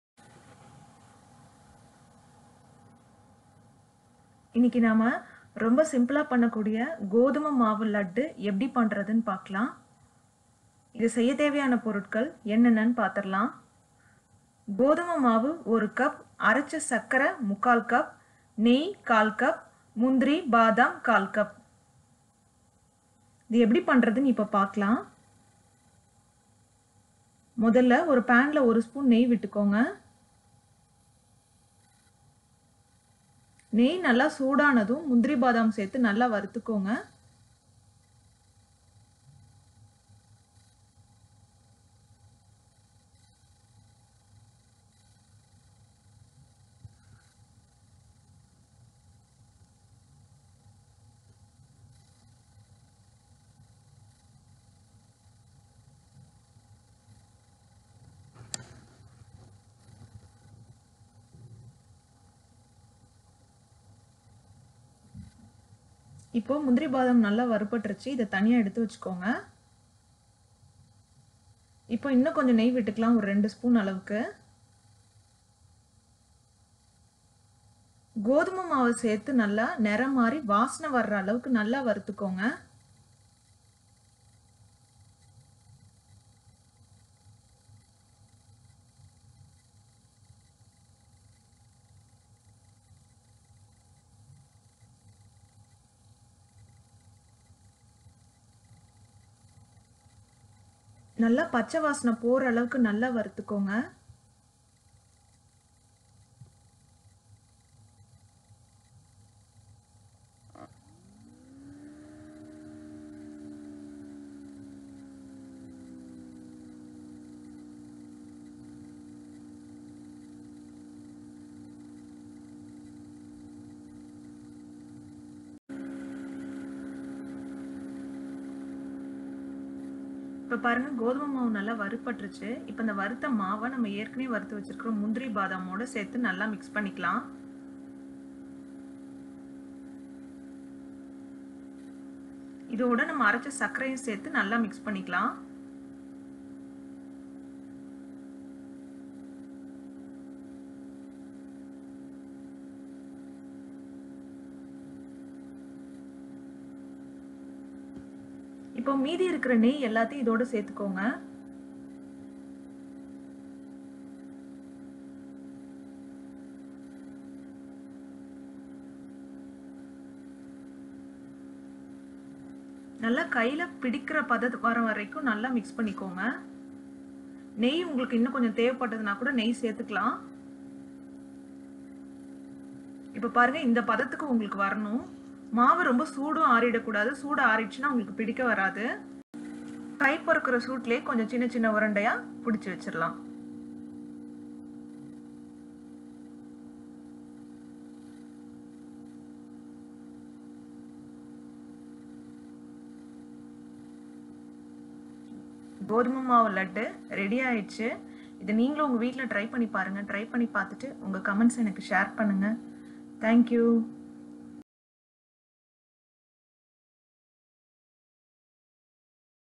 kodumai mavu laddu | கோதுமை மாவு லட்டு | Mathi's kitchen
This video explains the preparation of wheat flour laddu in tamil. Do try and post your comments